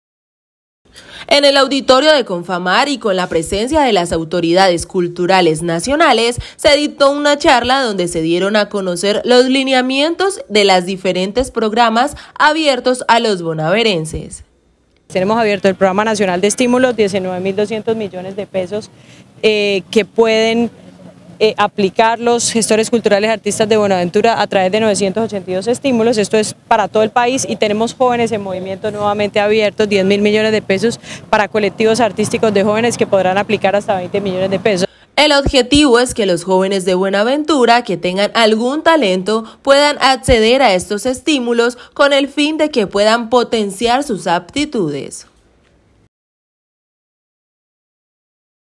CULTURA EN MOVIMIENTO
La convocatoria para el Programa Nacional de Estímulos ya está abierta, este programa busca apoyar a jóvenes gestores de cultura que se desempeñen en algún tipo de arte en el Distrito de Buenaventura.